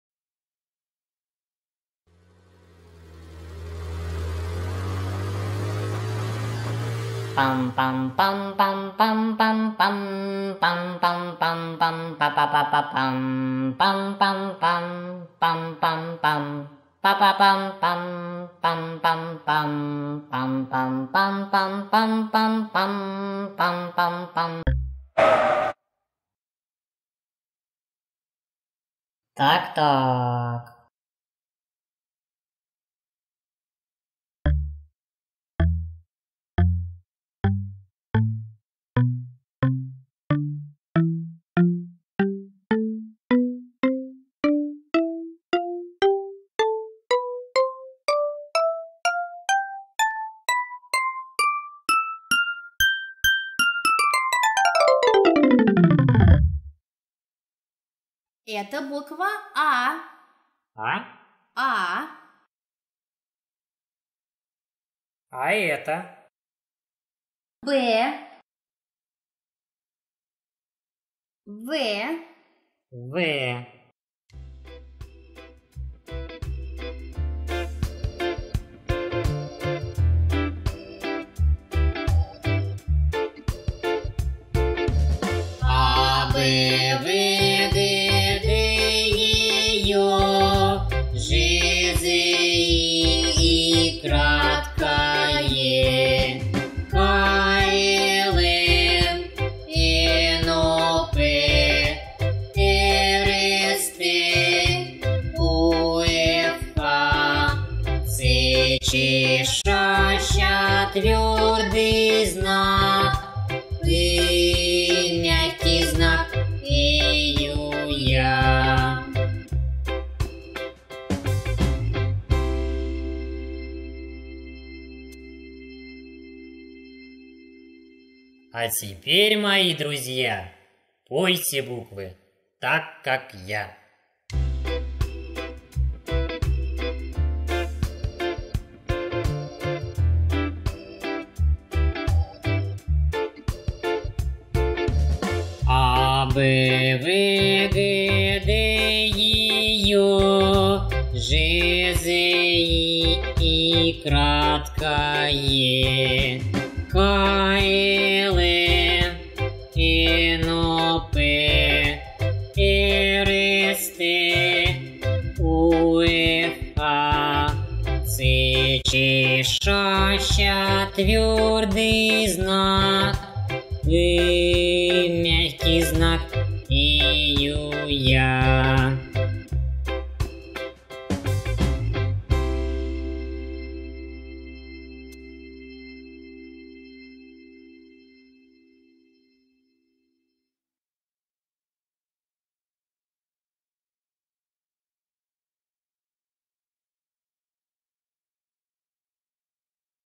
Учим алфавит с машинкой Развивающий мультик.
Учим буквы русского языка вместе с машинкой. В этом мультфильме маленькая машинка поможет малышам выучить русский алфавит.